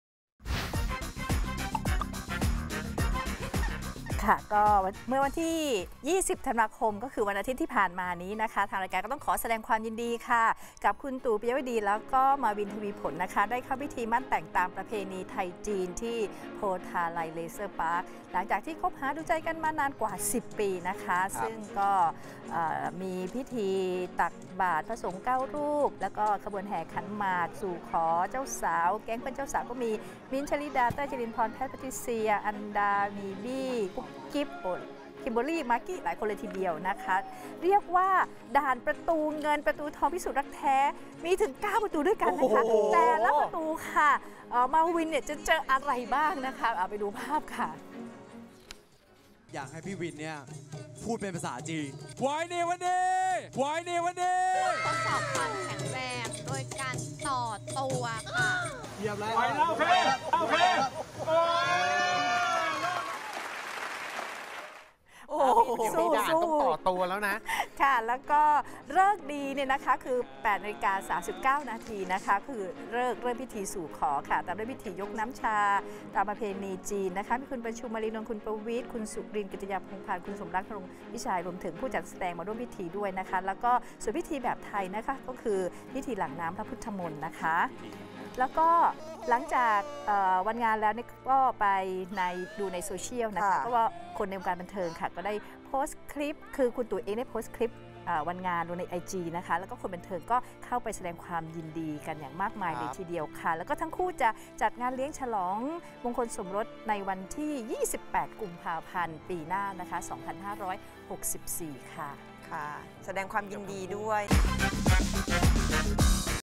ชื่นมื่นพิธีหมั้นและสมรส ผู้จัดตู่ ปิยวดี - มาวิน
มาวิน - ตู่ ปิยวดี เข้าพิธีหมั้นวิวาห์ชื่นมื่น คนบันเทิงร่วมยินดี เตรียมจัดฉลองมงคลสมรส 28 ก.พ.64